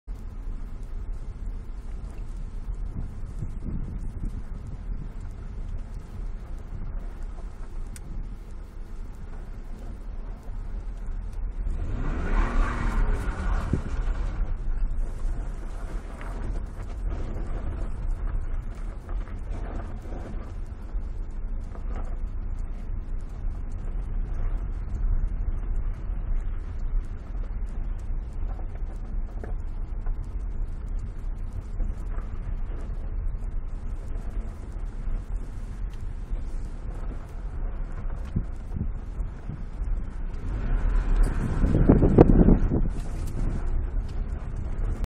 BMW 525i E39 manual in a snowy car park drifting ...sort of
In-car cam of BMW E39 in the snow going sideways. Well a bit sideways. Turn up for engine noise.